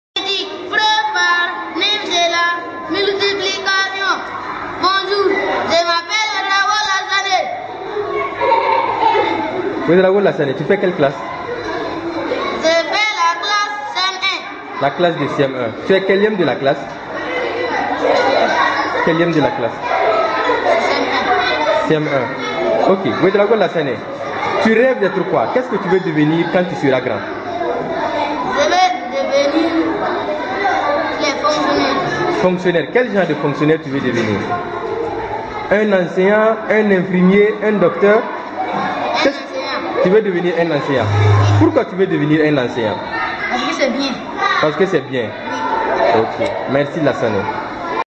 J'ai un rêve
Ouedraogo Lassane âgé de 14ans dans la classe de CM1 à l'école de Kayégue,rêve d'être un jour enseignant